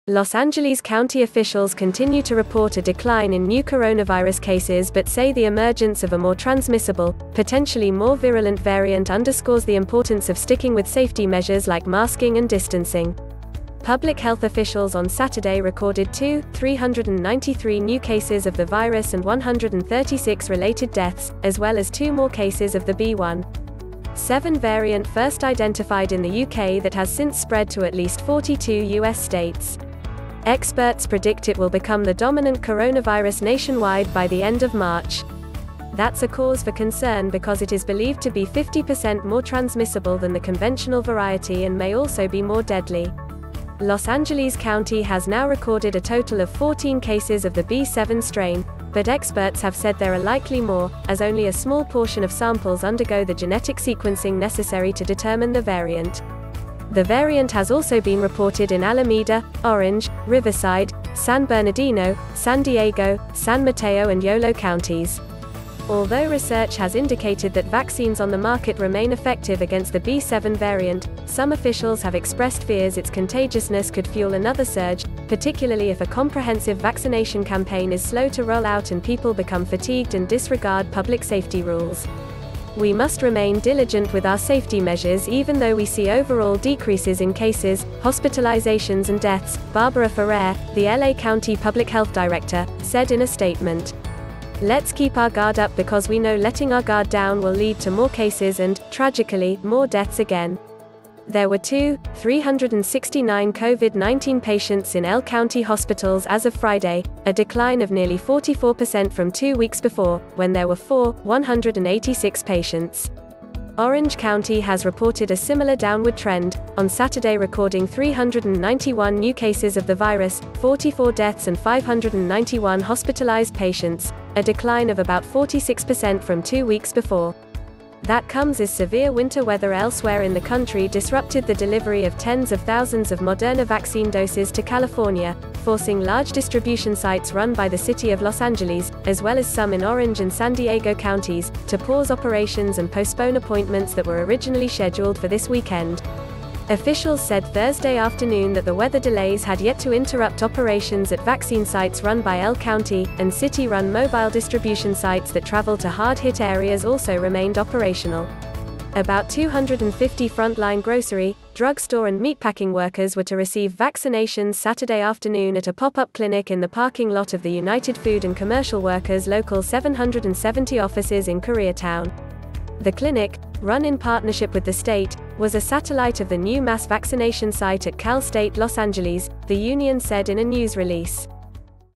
L A County coronavirus numbers continue to drop as more cases of U K
Los Angeles County officials continue to report a decline in new coronavirus cases but say the emergence of a more transmissible, potentially more virulent variant underscores the importance of sticking with safety measures like masking and distancing. Public health officials on Saturday recorded 2, 393 new cases of the virus and 136 related deaths, as well as two more cases of the B. 1. 7 variant first identified in the U. K. that has since spread to at least 42 U. S. states. Experts predict it will become the dominant coronavirus nationwide by the end of March. That’s a cause for concern because it is believed to be 50% more transmissible than the conventional variety and may also be more deadly. Los Angeles County has now recorded a total of 14 cases of the B. 7 strain, but experts have said there are likely more, as only a small portion of samples undergo the genetic sequencing necessary to determine the variant. The variant has also been reported in Alameda, Orange, Riverside, San Bernardino, San Diego, San Mateo and Yolo counties. Although research has indicated that vaccines on the market remain effective against the B. 7 variant, some officials have expressed fears its contagiousness could fuel another surge, particularly if a comprehensive vaccination campaign is slow to roll out and people become fatigued and disregard public safety rules.“We must remain diligent with our safety measures even though we see overall decreases in cases, hospitalizations and deaths, ” Barbara Ferrer, the L. A. County public health director, said in a statement. “Let’s keep our guard up because we know letting our guard down will lead to more cases and, tragically, more deaths again. ”There were 2, 369 COVID-19 patients in L. County hospitals as of Friday, a decline of nearly 44% from two weeks before, when there were 4, 186 patients. Orange County has reported a similar downward trend, on Saturday recording 391 new cases of the virus, 44 deaths and 591 hospitalized patients, a decline of about 46% from two weeks before. That comes as severe winter weather elsewhere in the country disrupted the delivery of tens of thousands of Moderna vaccine doses to California, forcing large distribution sites run by the city of Los Angeles, as well as some in Orange and San Diego counties, to pause operations and postpone appointments that were originally scheduled for this weekend. Officials said Thursday afternoon that the weather delays had yet to interrupt operations at vaccine sites run by L. County, and city-run mobile distribution sites that travel to hard-hit areas also remained operational. About 250 front-line grocery, drugstore and meatpacking workers were to receive vaccinations Saturday afternoon at a pop-up clinic in the parking lot of the United Food and Commercial Workers Local 770 offices in Koreatown.